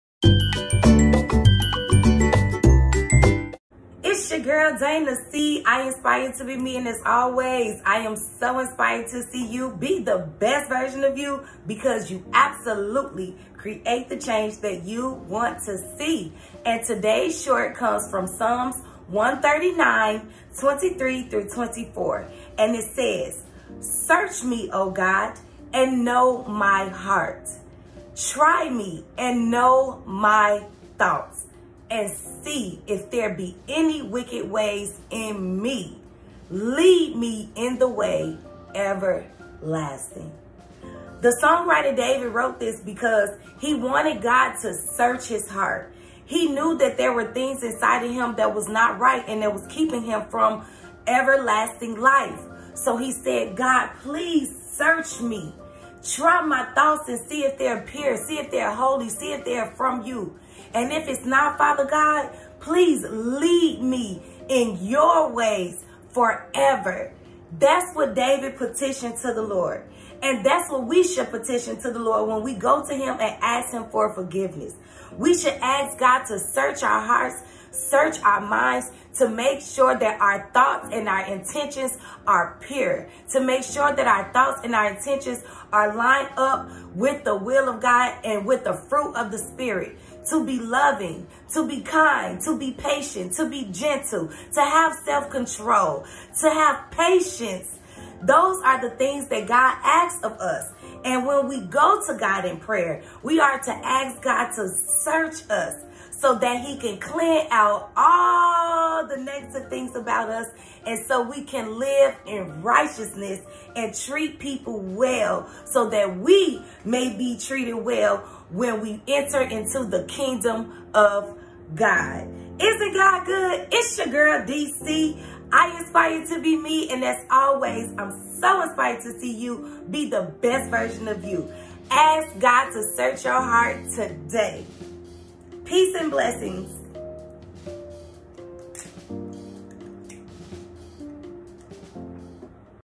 I’ve Been Looking For You
Psalm 139:23-24 teaches us to mindful that we are not perfect. Your right may not always be the correct right. Your wrong may not always be wrong. Ask God to help you figure it out.